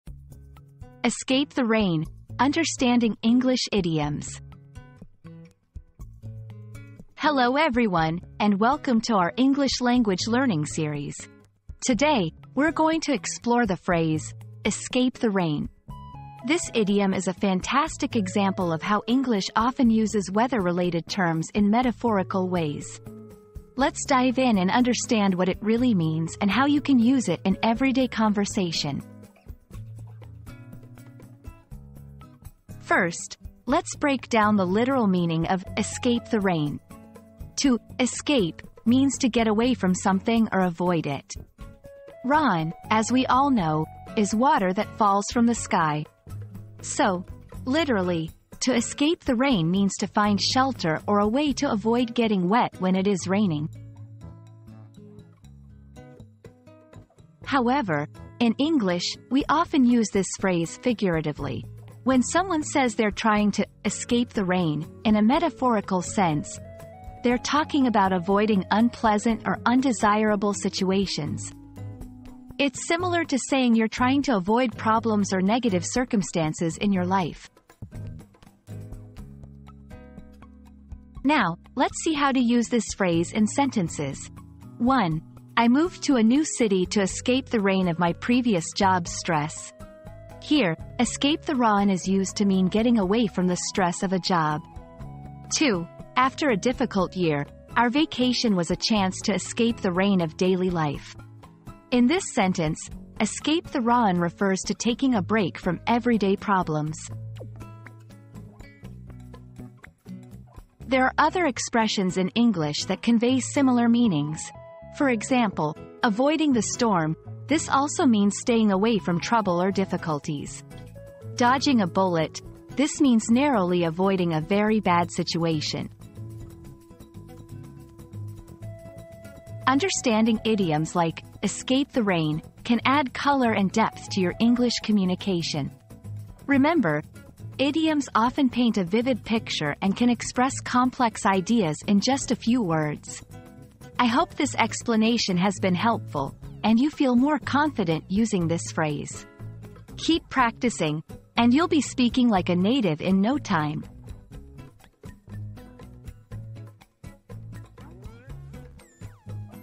Escape the Rain: Understanding English Idioms
00:00 • Introduction - Escape the Rain: Understanding English Idioms
00:32 • The Literal Meaning
00:56 • The Figurative Meaning
01:20 • Usage in Sentences
01:50 • Similar Expressions